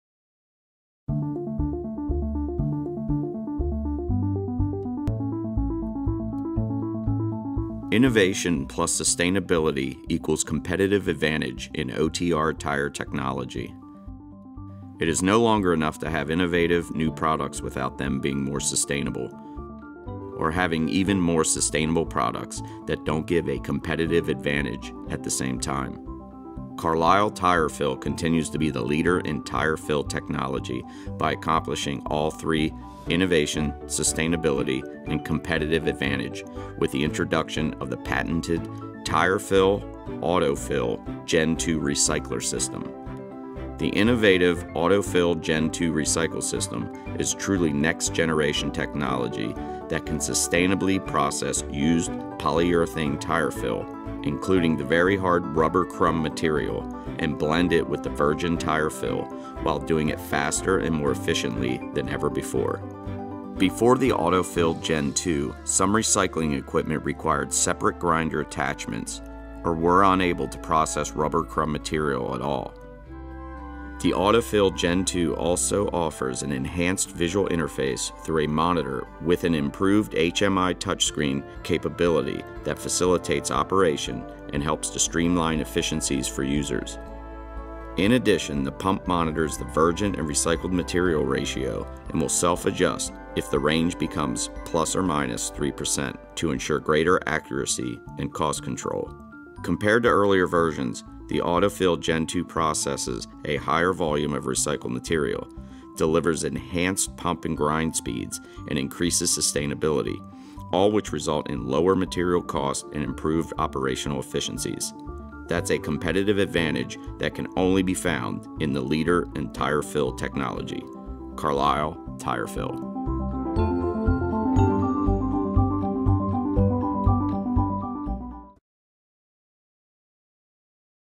The AutoFil-GenII - Innovation + Sustainability = Competitive Advantage
It is no longer enough to have innovative new products without them being more sustainable. Or having even more sustainable products that don't give a competitive advantage at the same time. Carlisle TyrFil continues to be the leader in tire fill technology by accomplishing all three, innovation, sustainability, and competitive advantage with the introduction of the patented TyrFil AutoFil-GenII Recycler System.